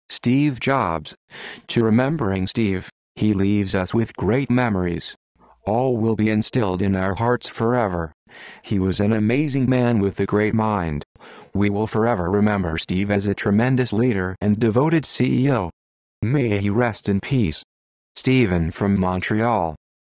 Remembering Steve #229
Steve Jobs
To remembering Steve,He leaves us with great memories. All will be instilled in our hearts forever. He was an amazing man with a great mind. We will forever remember Steve as a tremendous leader and devoted CEO. May he rest in peace.
Steven from Montreal